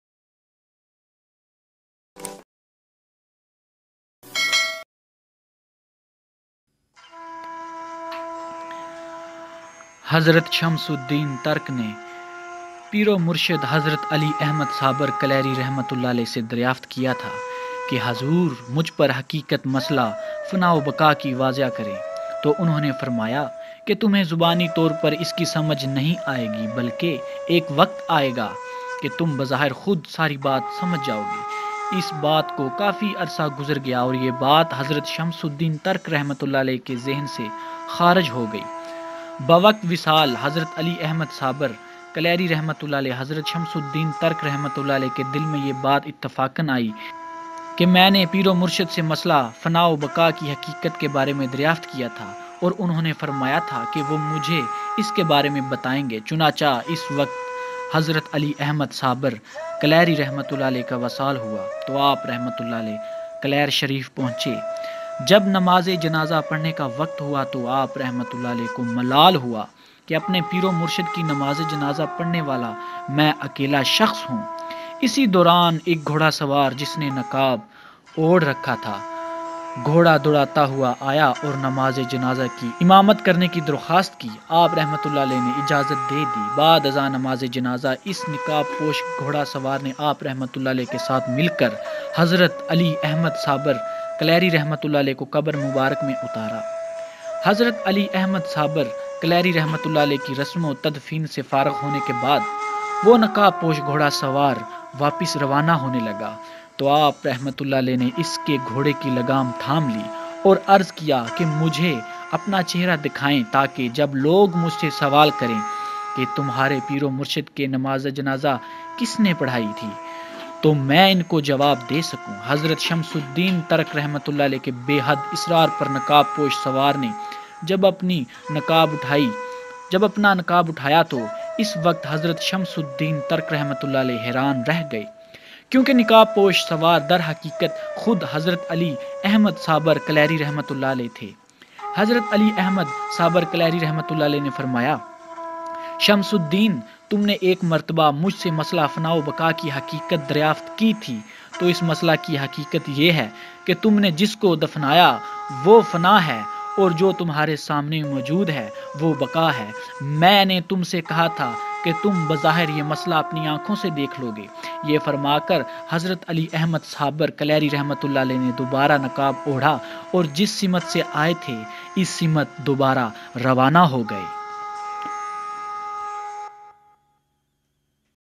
Fanaa aur baqaa ki Hakikat.NEW VIDEO 2020.Allah Walo Ki Baatein.
Allah walo ki baatein.Welcome To Allah walo ki baatein. Youtube Channel in this Video We Will Share a video about Fanaa aur baqaa ki Hakikat. or Hazrat Hassan Basri ka wakya - Islamic Short Stories With Moral in Urdu/Hindi We hope in This video give you a good Massage and we are also hope you are like this video.
Allah walo ki baatein.